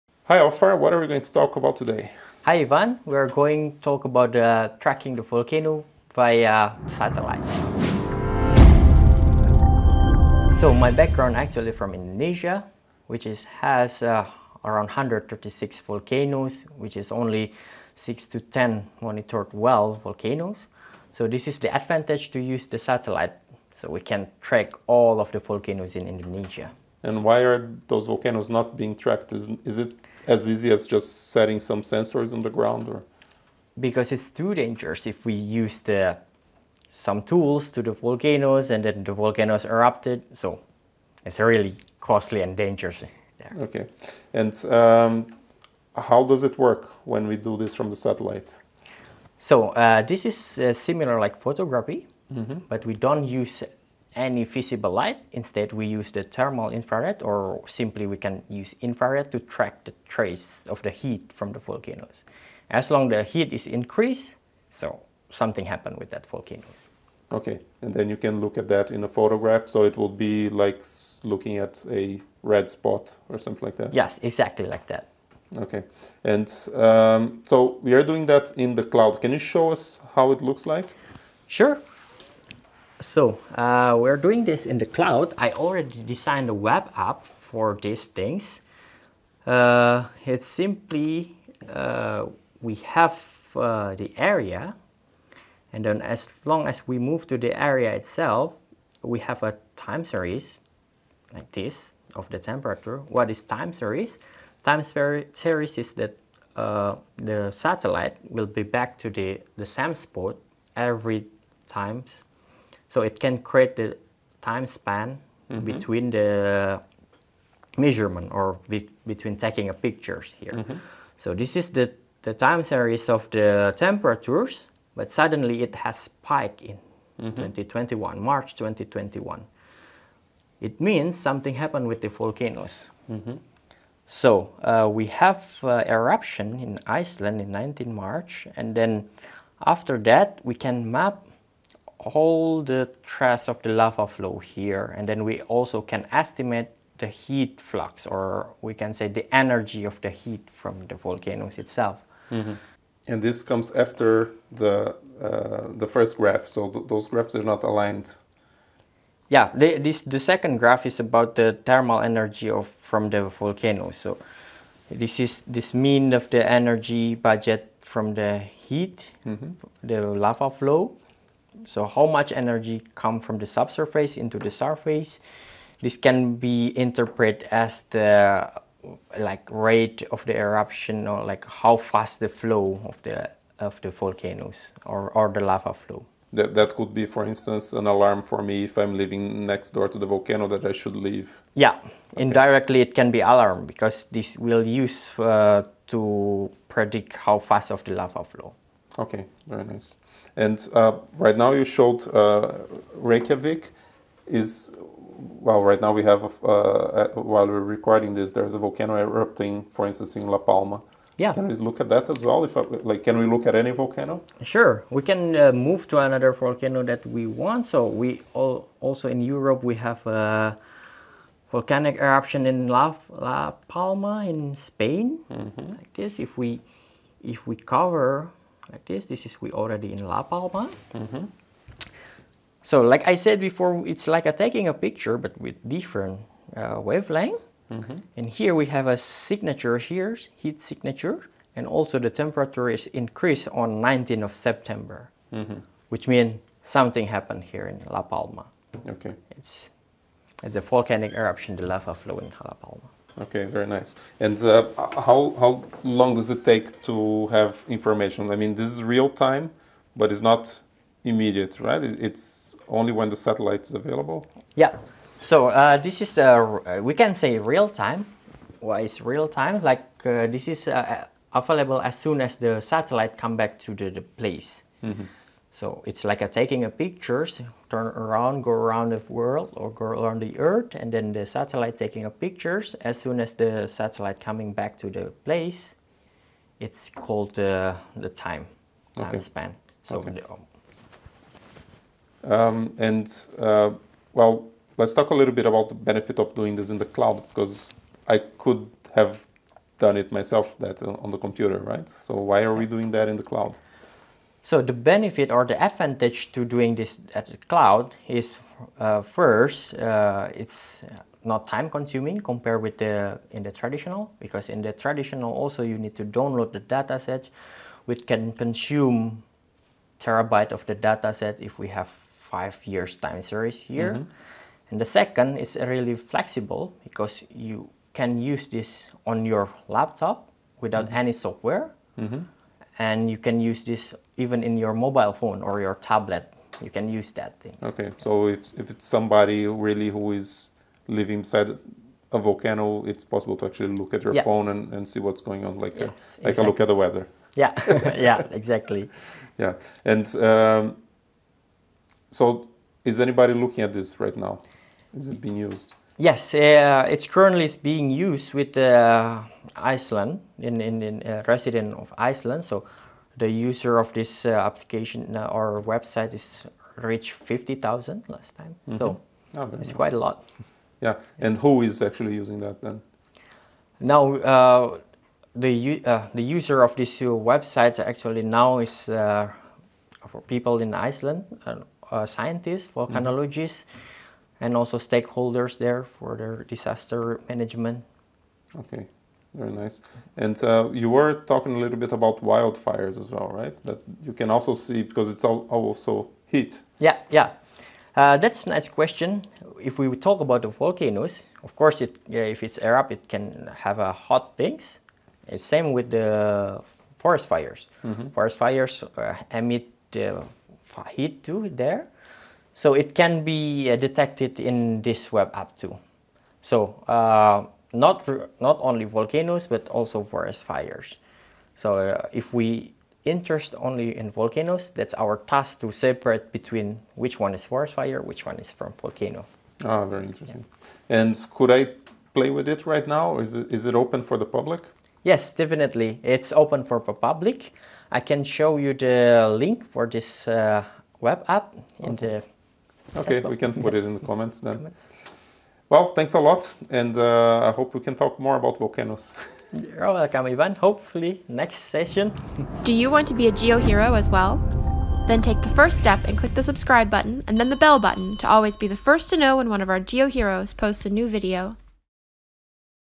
Tracking volcanoes from the sky - GeoHero
Dr. Mohammed Aufaristama explains how he uses Google Earth Engine to track volcanoes activity in real-time using satellite images.